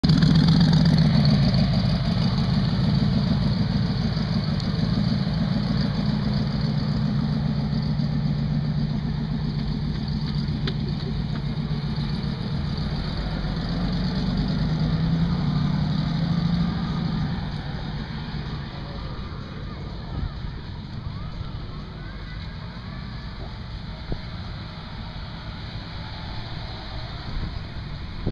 Cars Leave the 2013 Eyes On Design Car Show Part 1 - CarsInDepth.com 3D Video
John Mayer's 1935 Ford Phaeton "Fearless" (2013 Great 8 Ridler Finalist), 1936 Cadillac 8-75 Fleetwood convertible sedan, 1954 Hudson Jet convertible prototype